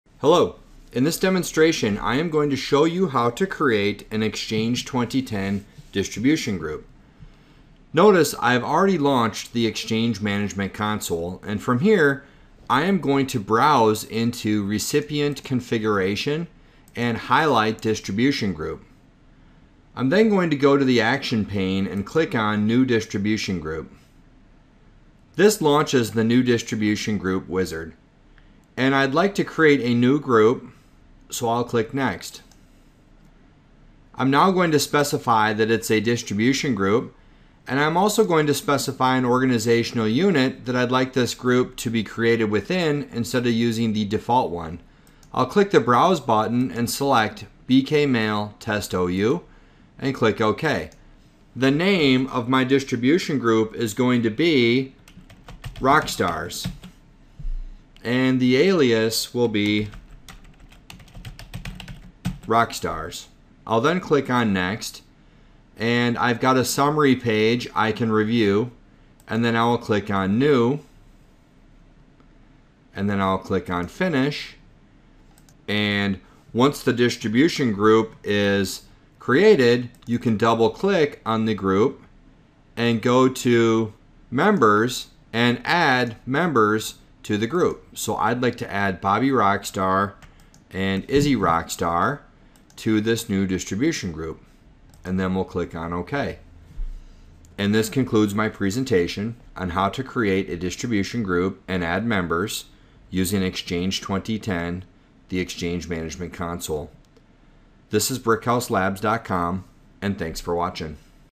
Create Exchange 2010 Distribution Group
Learn how to create an Exchange 2010 Distribution Group (List) with this easy tutorial.